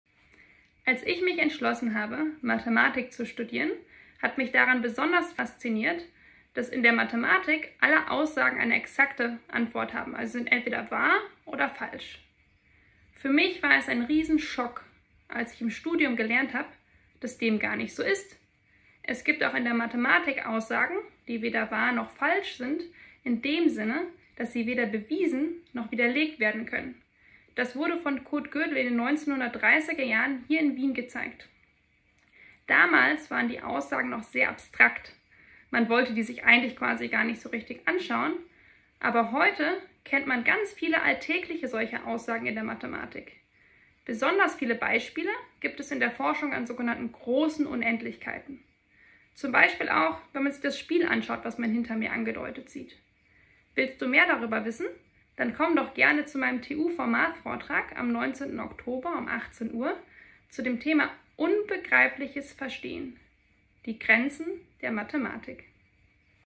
Unbegreifliches verstehen — die Grenzen der Mathematik (TUForMath Vortragsankündigung Sandra Müller)
Ankündigung TUForMath Vortrag Wintersemster 2023/24 Sandra Müller, TU Wien